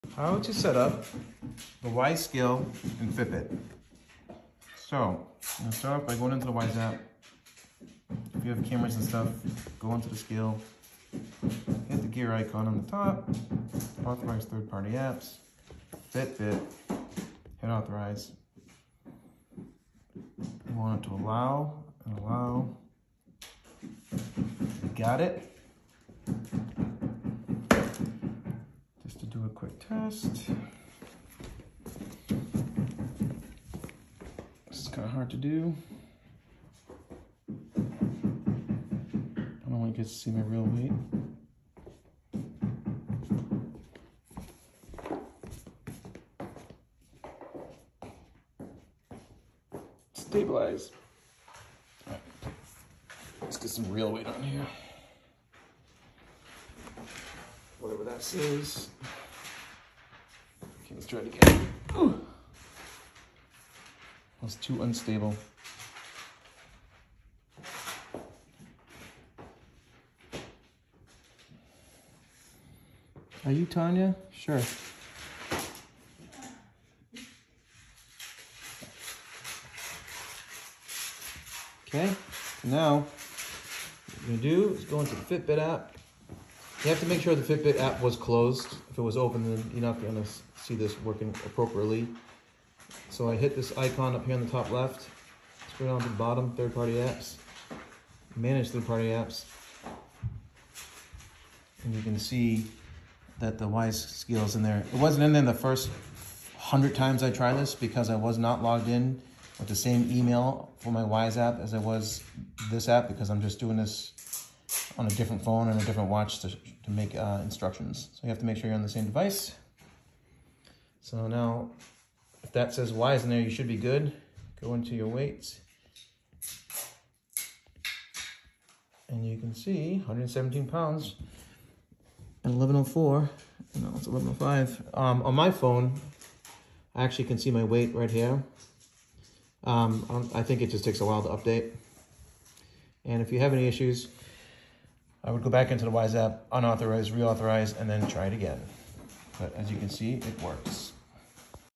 How to setup the Wyze scale in the Fitbit app
Instructions on how to synch the Wyze scale with Fitbit.